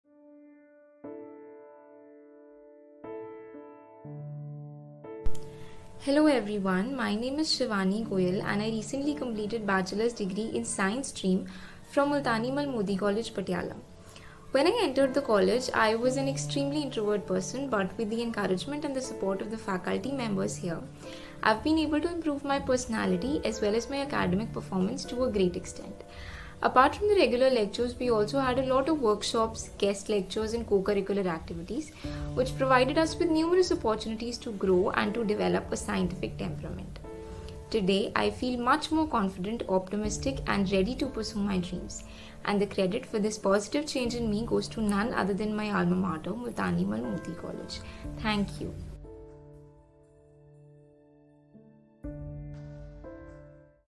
Students Speak - Shivani Goyal
It really matters what student speaks about your institution. Here is what a science student Shivani Goyal has to say about Multani Mal Modi College, Patiala. (Video #3)